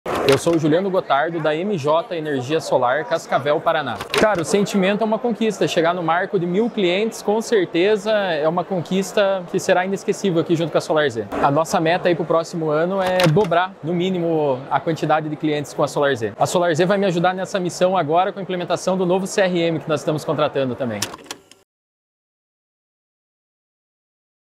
Depoimento - Integradores Parceiros SolarZ: MJ Energia
Durante a realização da última Intersolar South America 2024, maior evento de energia solar da América Latina, nós prestigiamos os nossos integradores parceiros com placas de reconhecimento pela quantidade de usinas solares administradas e monitoradas pelas tecnologias da SolarZ.

Neste vídeo, nosso parceiro Juliano Gotardo, da MJ Energia, expressa a sua emoção pela placa de mais de 1 mil usinas.

“O nosso objetivo é dobrar a meta para o ano que vem e a SolarZ vai nos ajudar agora com a implementação do novo CRM”, destaca Juliano.

Quer seguir o exemplo da MJ Energia e fazer parte dessa história de sucesso junto com a SolarZ? Acesse o nosso site, preencha seu cadastro, fale com o nosso time comercial e invista no futuro do seu negócio!